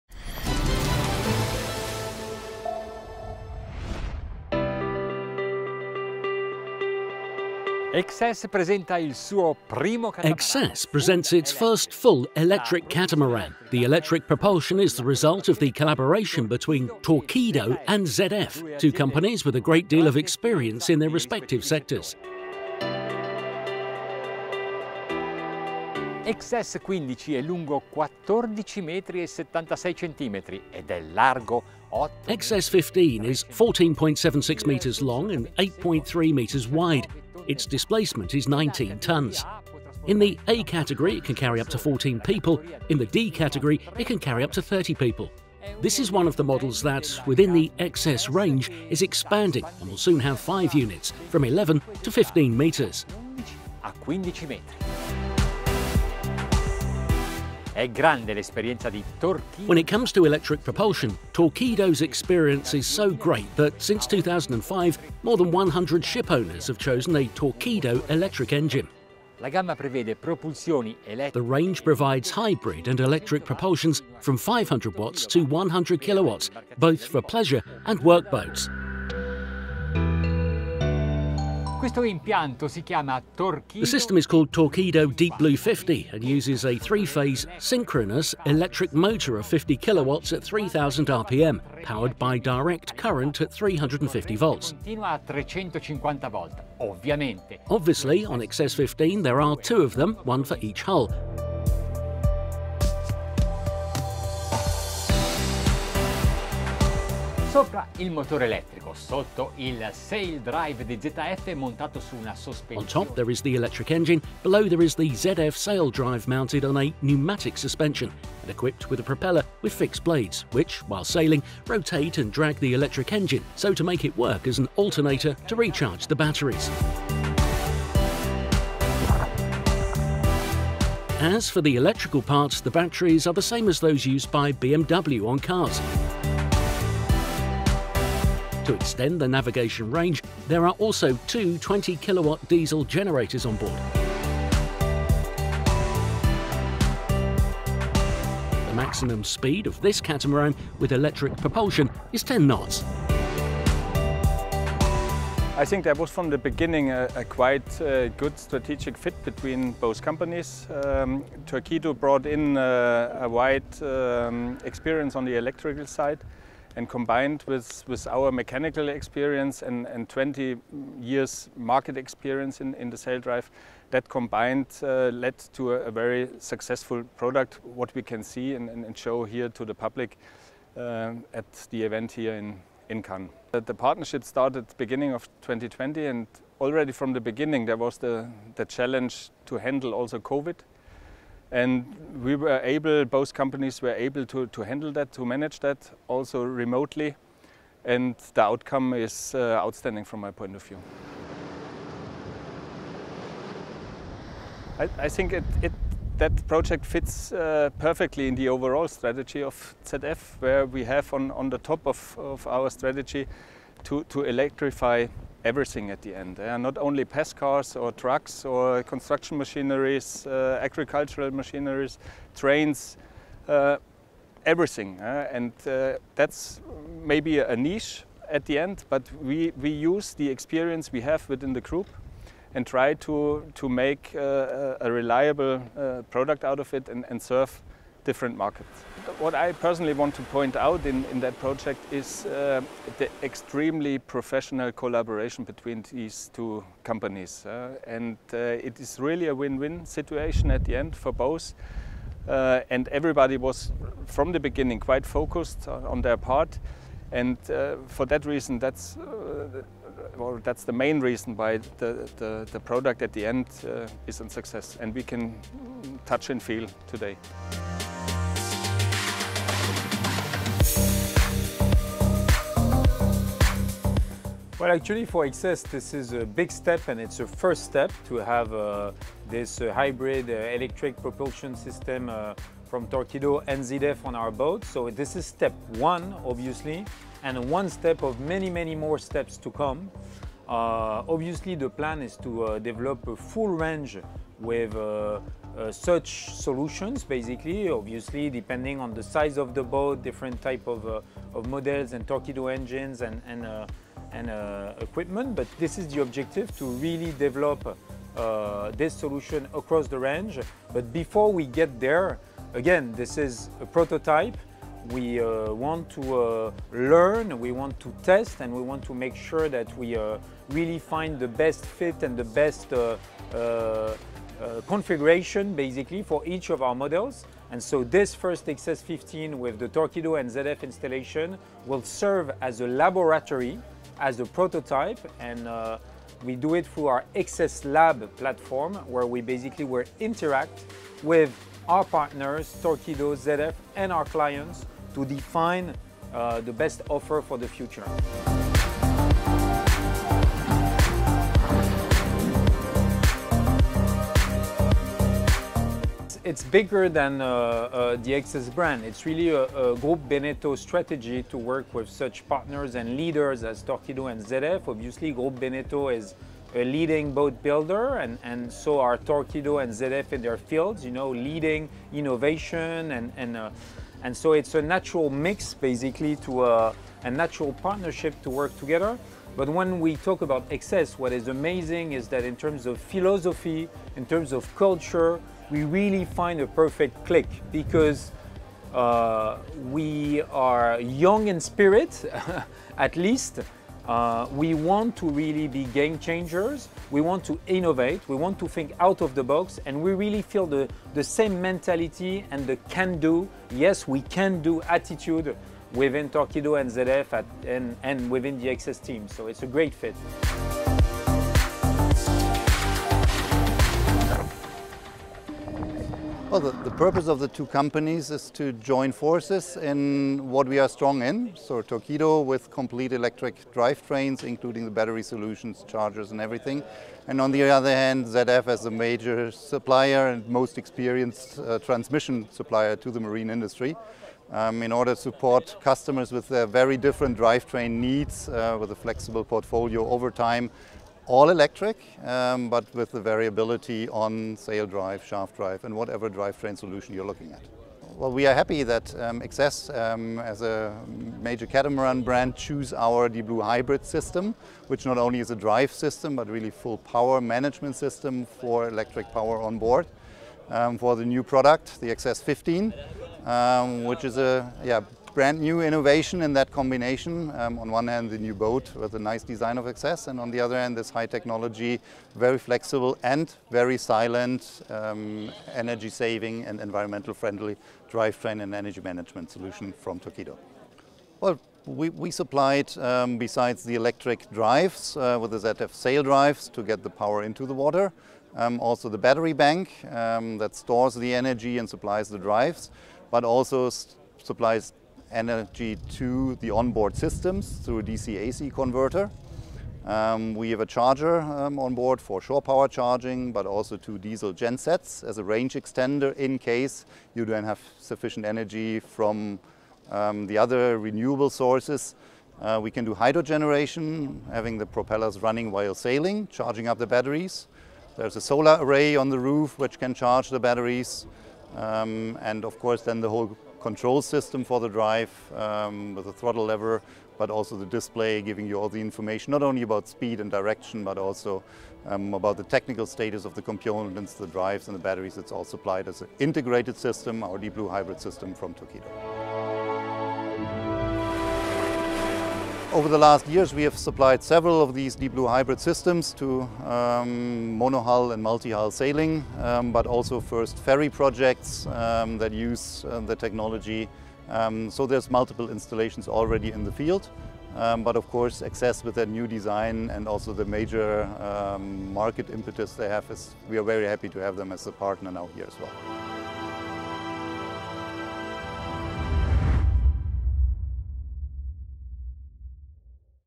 [ENG] FULL ELECTRIC SAILING CATAMARAN - Review Propulsion by ZF and Torqueedo - The Boat Show
By Maurizio Bulleri
Location: Cannes, France